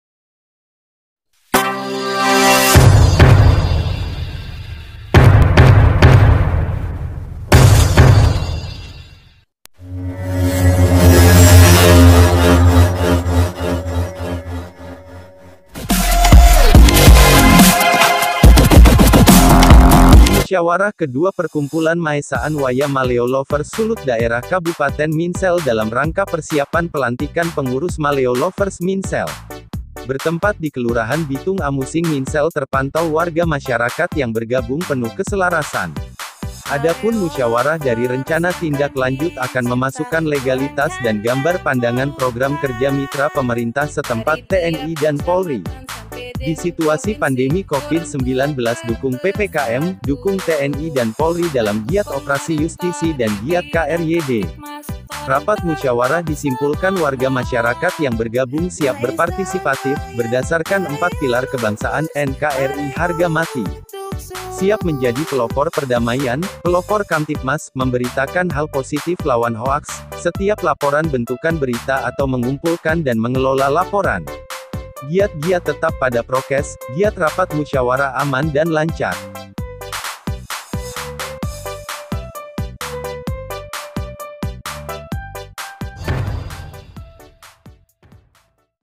MW Maleo Lovers Minsel Rapmus ke - 2 persiapan pelantikan sebagai pelopor kamtibmas
Musyawarah ke dua Perkumpulan Maesa'an Waya Maleo Lovers Sulut daerah Kabupaten Minsel dalam rangka persiapan pelantikan pengurus Maleo Lovers Minsel

Bertempat di kelurahan bitung Amusing Minsel terpantau warga masyarakat yang bergabung penuh keselarasan.

Adapun musyawarah dari rencana tindak lanjut akan memasukan legalitas dan gambar pandangan program kerja mitra pemerintah setempat, TNI dan Polri.

Di situasi pandemi covid - 19 dukung PPKM, dukung TNI dan Polri dalam giat oprasi Yustisi dan giat KRYD.

Rapat musyawarah disimpulkan warga masyarakat yang bergabung siap berpartisipatif, berdasarkan 4 pilar kebangsaan,NKRI harga mati.

Siap menjadi pelopor perdamaian, pelopor kamtibmas, memberitakan hal positif lawan hoaks, setiap laporan bentukan berita Atau mengumpulkan dan mengelola laporan.

Giat giat tetap pada prokes,giat rapat musyawarah aman dan lancar.